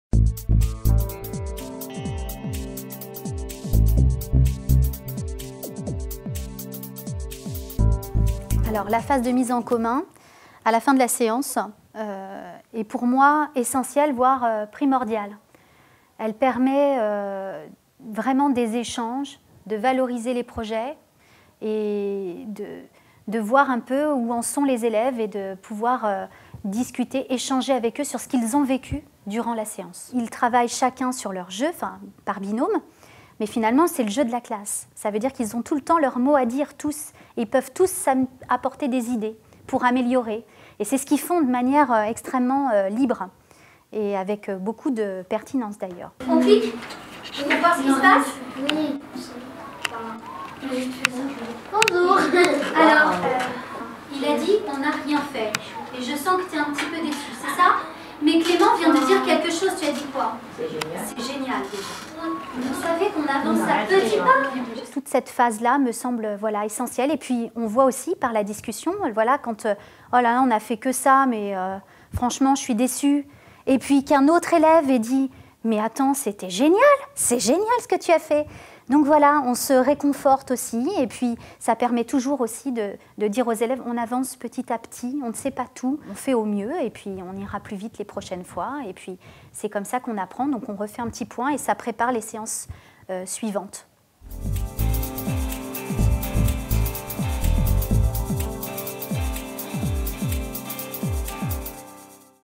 MOOC - Conversion numérique - Phase de mise en commun CM-7/8
MOOC "Éducation par la recherche : conversion numérique  à l'École".

Partie design pédagogique.

Catherine Massicot, professeur des écoles